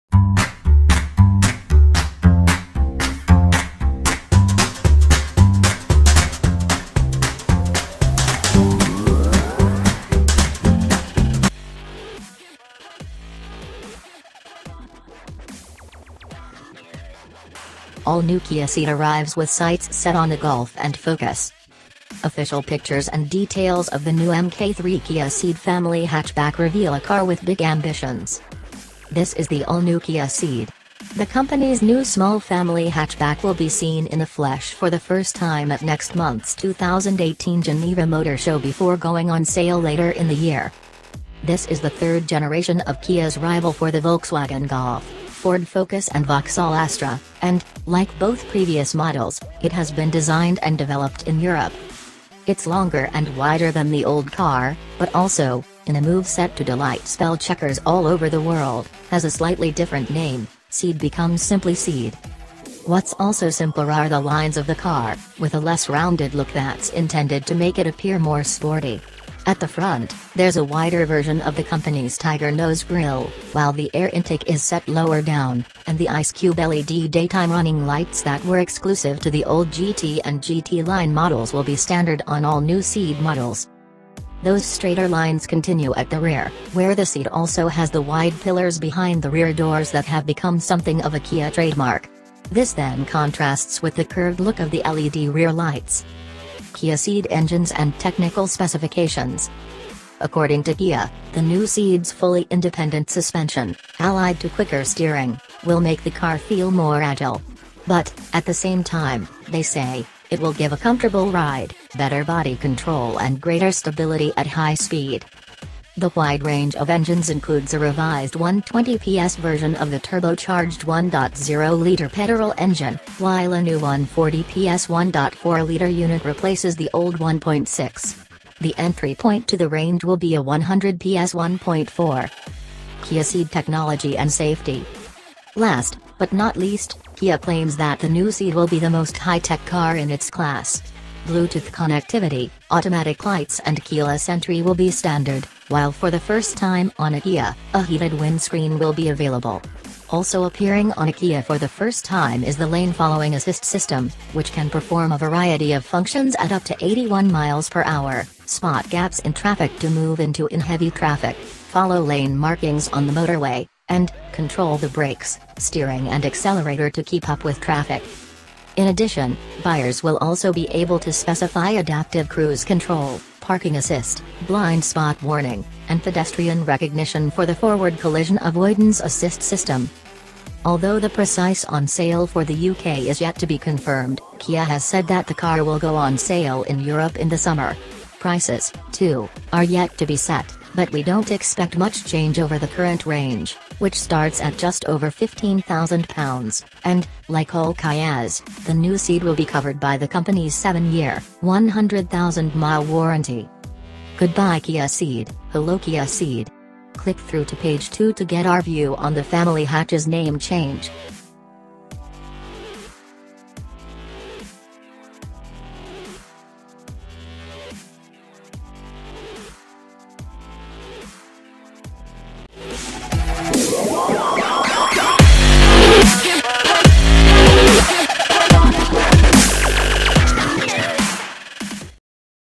All new Kia Ceed arrives with sights set on the Golf and Focus
Official pictures and details of the new mk3 Kia Ceed family hatchback reveal a car with big ambitions
This is the all-new Kia Ceed. The company’s new small family hatchback will be seen in the flesh for the first time at next month’s 2018 Geneva Motor Show before going on sale later in the year.

This is the third generation of Kia’s rival for the Volkswagen Golf, Ford Focus and Vauxhall Astra; and, like both previous models, it has been designed and developed in Europe.